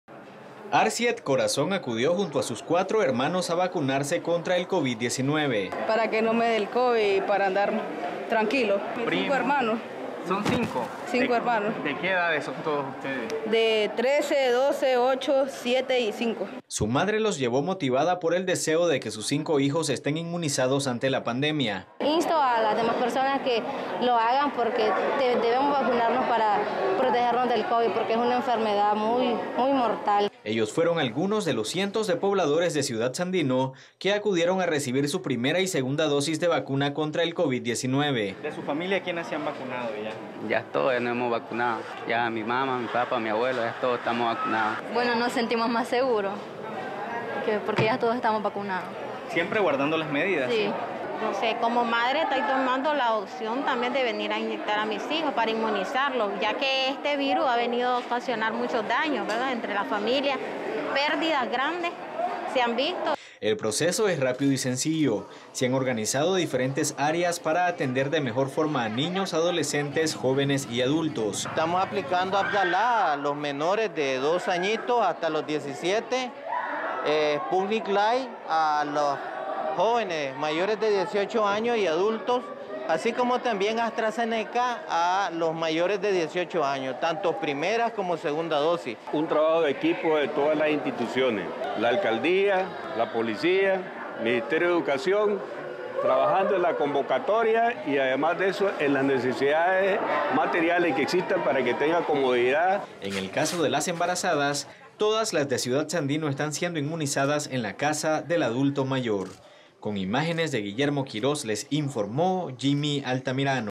Familias se continúan inmunizando contra el COVID-19 en Ciudad Sandino - Nicaragua
Este viernes se desarrolló la Jornada de Vacunación Voluntaria contra el COVID-19 en el Hospital Primario de Ciudad Sandino, donde fueron inmunizadas personas de diversas edades.

Arcied Corazón acudió junto a sus cuatro hermanos a vacunarse contra el COVID-19.

“Para que no me de el covid y andar tranquilo con mis hermanos”; dijo el jovencito de 13 años, quien es el mayor de cinco hermanos. El resto tienen 12, 8, 7 y 5 años de edad.

Su madre los llevó motivada por el deseo de que sus cinco hijos estén inmunizados ante la pandemia.

“Insto a las demás personas a que lo hagan. Debemos vacunarnos para protegernos contra el covid porque es una enfermedad mortal”; expresó Fátima Hernández habitante de Ciudad Sandino,

Ellos fueron algunos de los cientos de pobladores de Ciudad Sandino que acudieron a recibir su primera y segunda dosis de vacuna contra el COVID-19.

El joven Jonathan Hernández comentó que al vacunarse él todas en su familias están inmunizados, como son; su papá, mamá y abuelos.

En tanto, la adolescente Yakarely Quiroz mencionó que con la vacunación se siente más segura; pero seguirá tomando todas las medidas.

La madre de familia, Margarita Lara de Ciudad Sandino, explicó que tomó la opción de vacunar a sus hijos para inmunizarlos; ya que el virus ha ocasionado muchos daños a las familias del mundo y de Nicaragua. Pues pérdidas grandes se han visto.

Atención de calidad en Ciudad Sandino
El proceso es rápido y sencillo. Se han organizado diferentes áreas para atender de mejor forma a niños, adolescentes, jóvenes y adultos.

“Estamos aplicando Abdalá a los menores de 2 añitos hasta los 17, Sputnik Light a los jóvenes mayores de 18 años y adultos, así como también como AstraZeneca a los mayores de 18 años. Tanto primera como segunda dosis”; destacó el director del Hospital Primario de Ciudad Sandino, Dr. Manuel Largaespada.

“Un trabajo de quipo de todas las instituciones. La alcaldía, la policía, MINED, trabajando en la convocatoria y en las necesidades materiales que existan para darles comodidad”; puntualizó el alcalde de Ciudad Sandino, Raymundo Flores.

En el caso de las embarazadas, todas las Nicaragua están siendo inmunizadas contra el COVID-19.